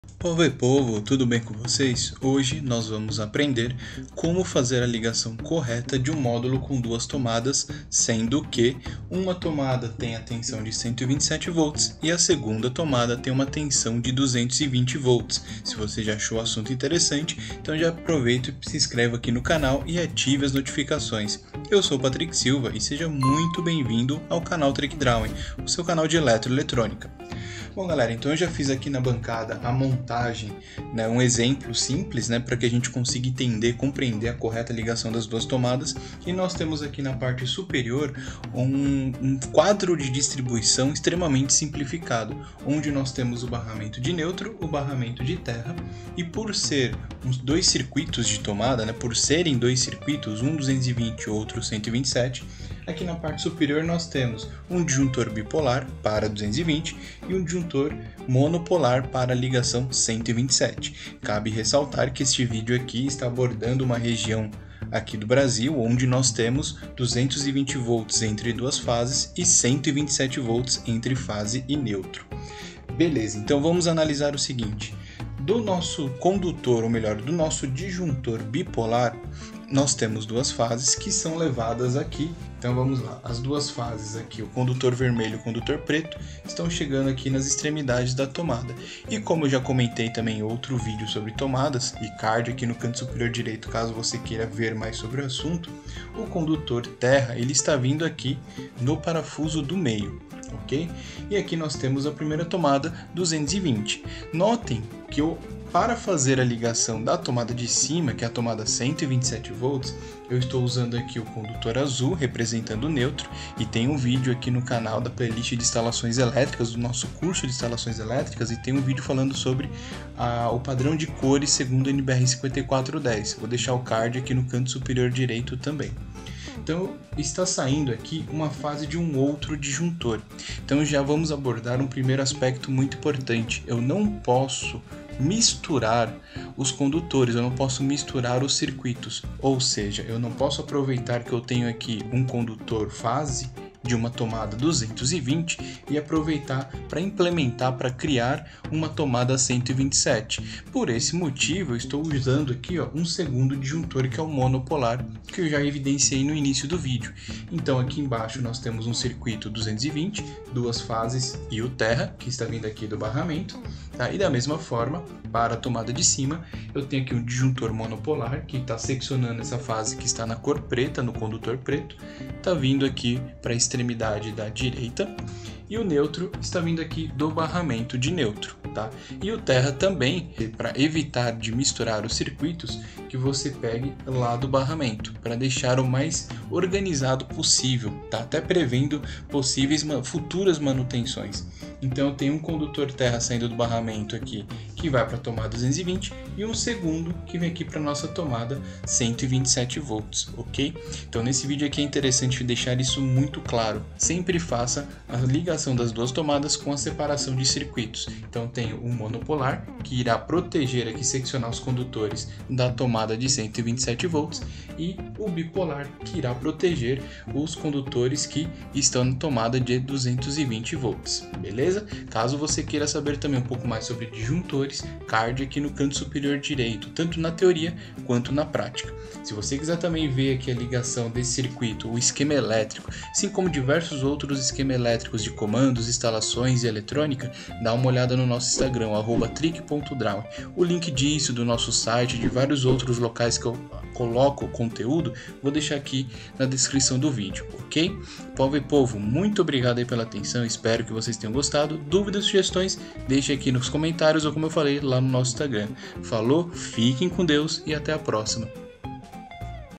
Você sabe instalar Tomada dupla 127V e 220V de forma correta ? Aprenda agora !
É comum encontrarmos em uma instalação elétrica, ambientes em um residência com módulos com duas ou mais tomadas, ou módulos com interruptor simples e tomada, módulo com interruptores simples e paralelos. As possibilidades de combinação são enormes por conta da facilidade.

Encontramos também módulos que apresentam uma ou duas conexões USB para carregar smarthphone.

Por conta dessa facilidade encontrada para instalações elétricas residenciais e comerciais se faz necessário que o profissional na área da elétrica saiba como instalar esses módulos garantindo a segurança dos moradores.  E a melhor e mais eficaz forma está em seguir as normas como a NBR 5410 para instalações elétricas de baixa tensão.

Nesta aula vamos apresentar como fazer a instalação de um módulo com duas tomadas sendo que uma é 127V e outra 220V levando em consideração a NBR 5410 e a NBR 14136.

Espero que esta aula do nosso curso de instalações elétricas possa contribuir com seu aprendizado.

Bons estudos !!!